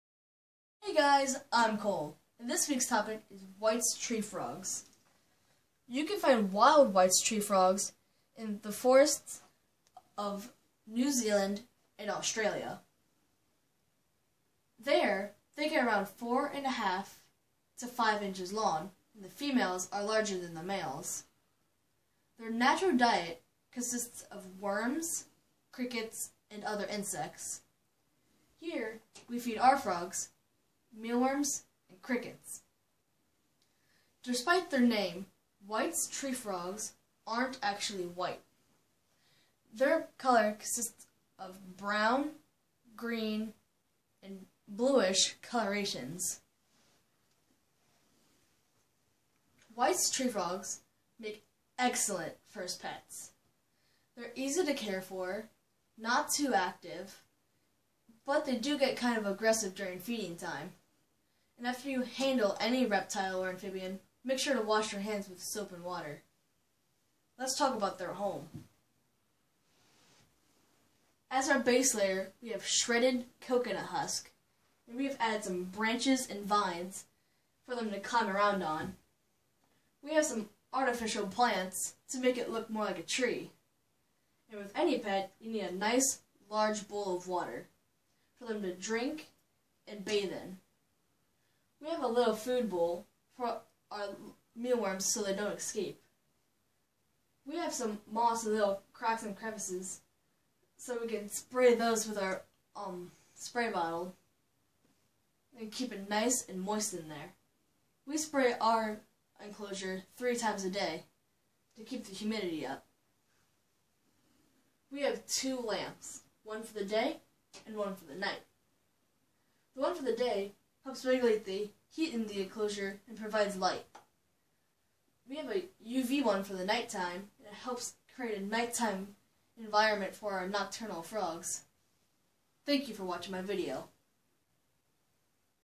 Whites Tree Frogs
I got this pair of White's Tree Frogs and they are pretty cool; the male will croak if he hears water running in the pipes or when we are vacuuming the room.